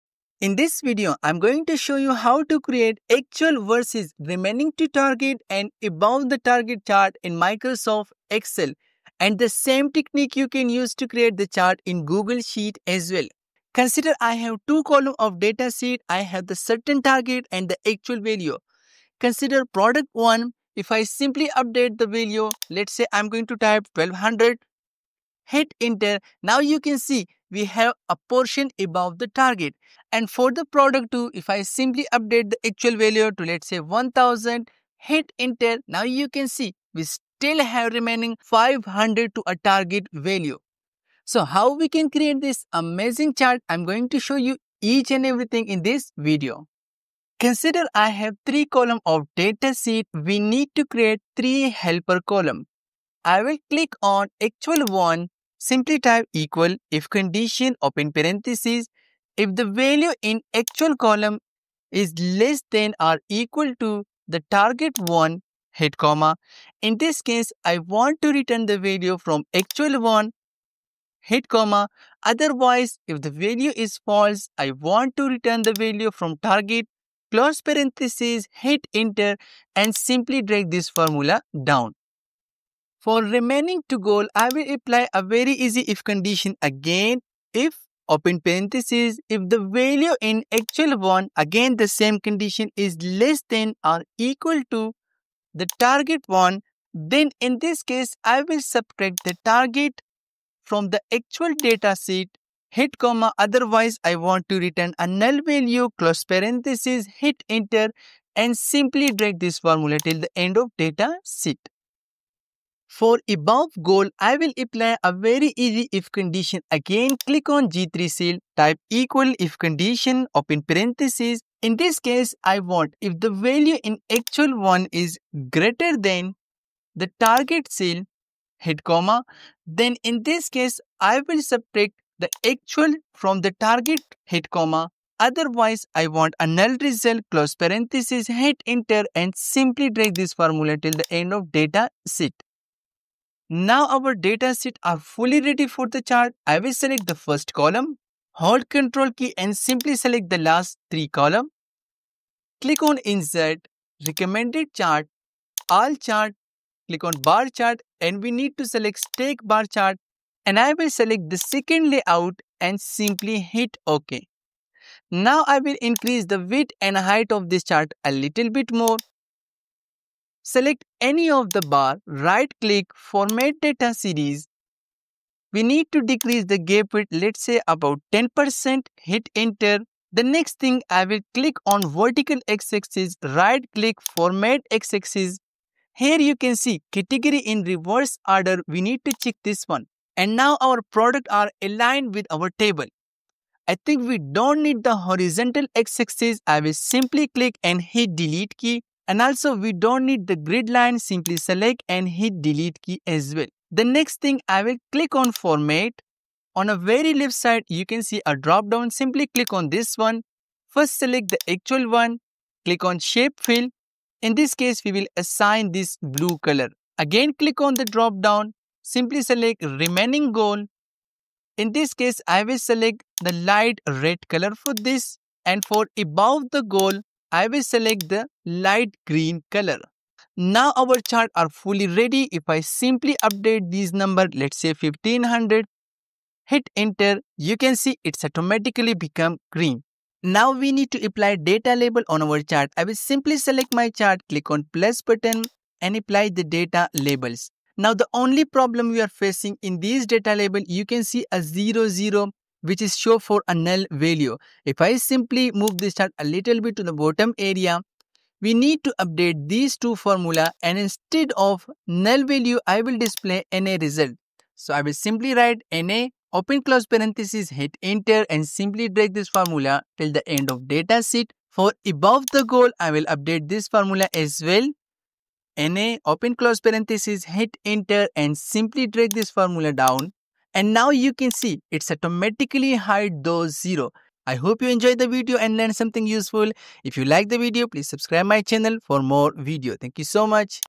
Track Actual vs Target Performance: Remaining & Above with Excel Bar Chart
In this tutorial, learn how to track Actual vs Target performance using Excel bar charts. We’ll show you how to display remaining and above-target values to better understand your data. This step-by-step guide will teach you how to effectively track Actual vs Target in Excel and analyze your performance at a glance.

*=CHAPTER=*
0:00 Intro about chart
0:46 Calculation
2:23 Inserting a Chart and Formatting